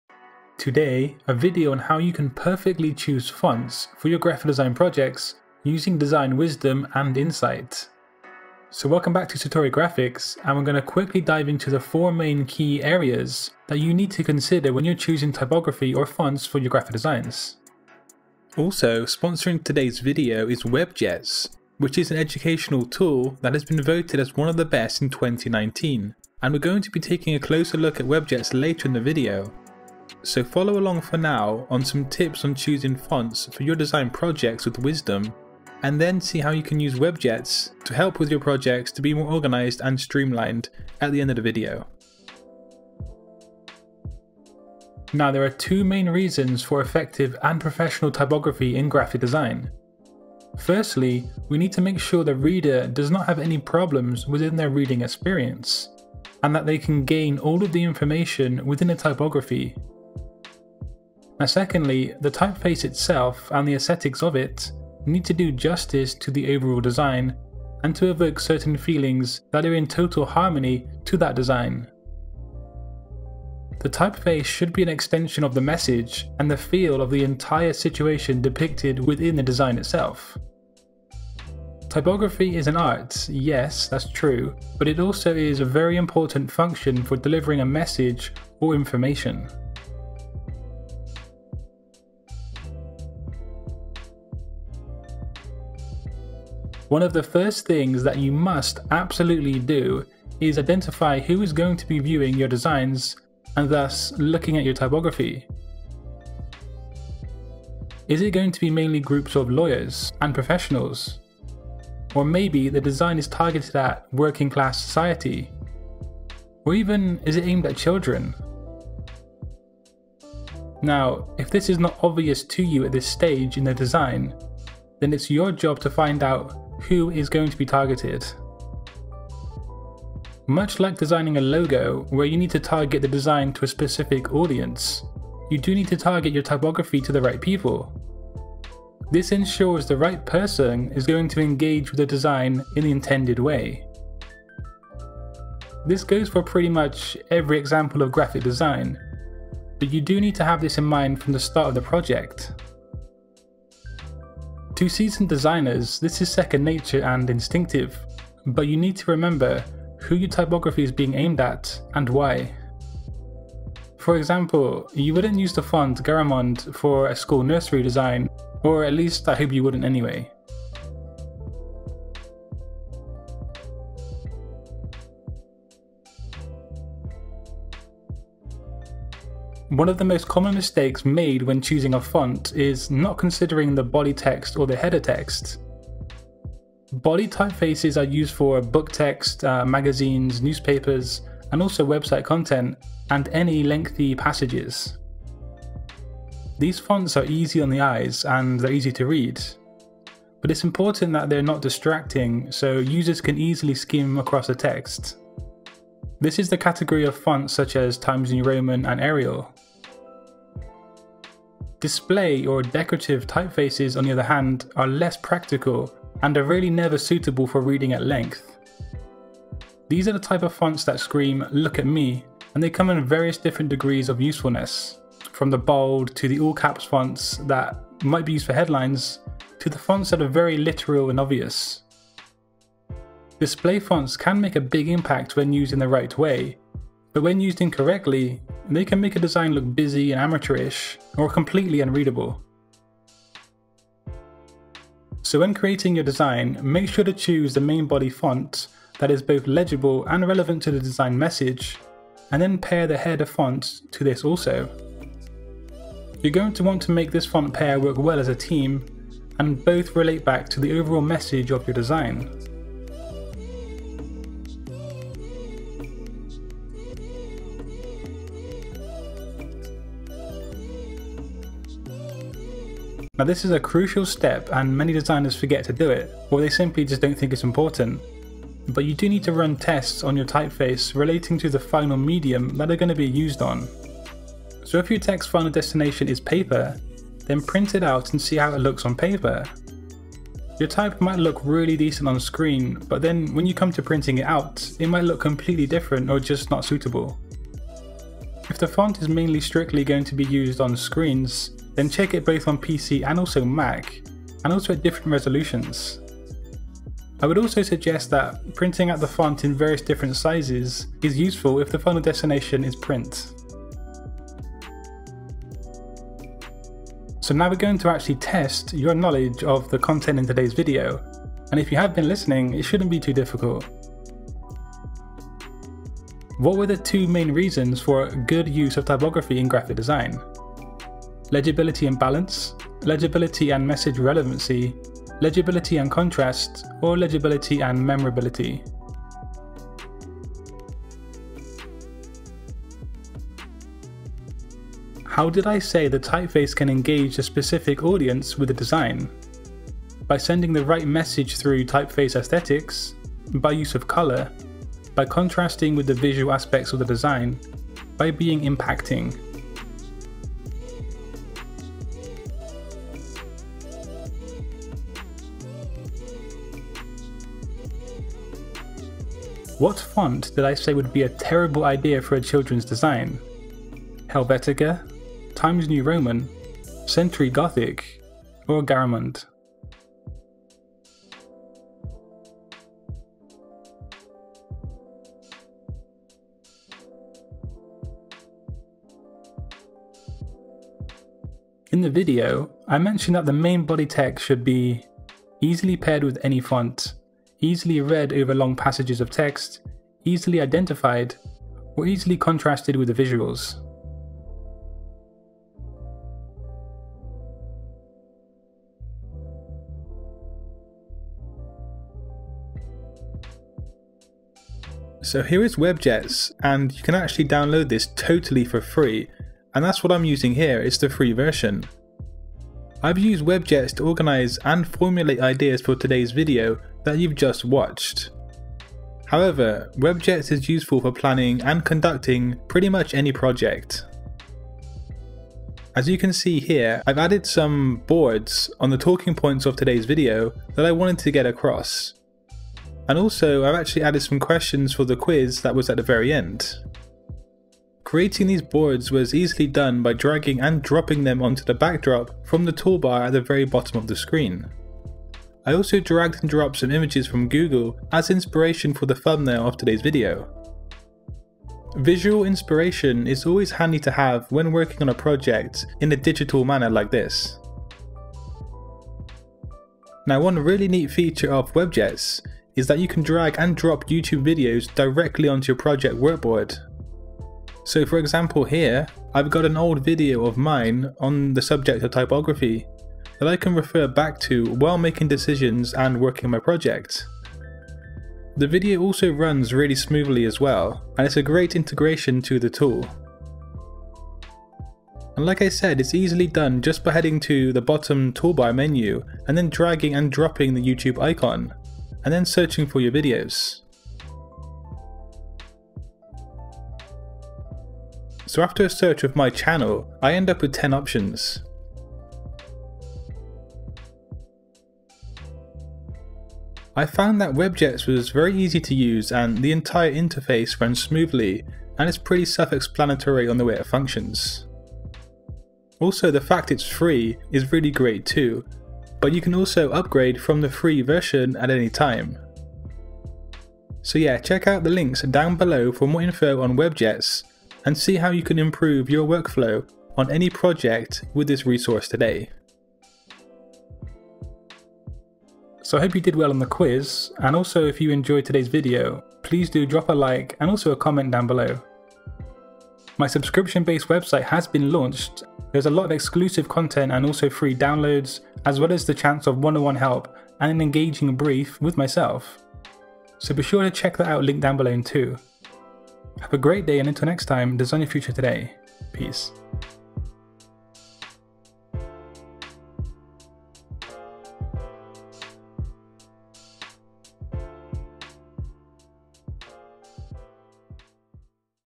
How To Choose Fonts With Design Wisdom *PRO TIPS*
How to choose fonts for your designs, with wisdom!

Choosing fonts for your designs can be daunting, and it’s a fact that it’s one of the most crucial aspects of your design to focus on. In todays tutorial I aim to show you some points and tips on selecting fonts for your projects. There is even a mini quiz at the very end of the video, to test your knowledge on the content in the video, to see if you have absorbed the information on choosing fonts for your graphic designs. Let me know what you thought of todays content, do you agree with my points and tips on selecting fonts?

MUSIC